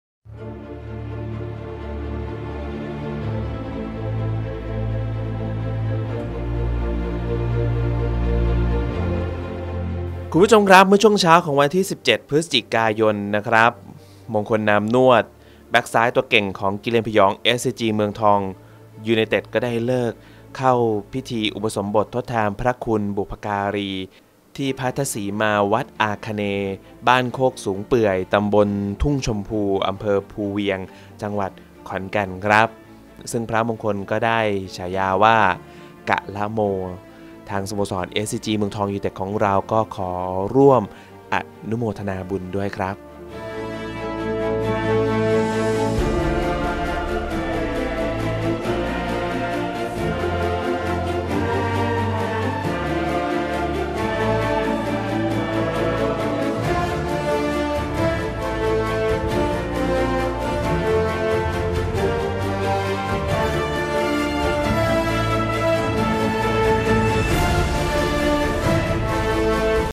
MTUTD.TV มงคล นามนวด อุปสมบท
MTUTD.TV มงคล นามนวด เข้าอุปสมบททดแทนพระคุณ บิดา มารดา วันที่ 17 พ.ย.55 ที่ พัทธสีมา วัดอาคเนย์ บ้านโคกสงเปือย ต.ทุ่งชมพู อ.ภูเวียง จ.ขอนแก่น ได้ฉายาว่า "กะละโม "ครับ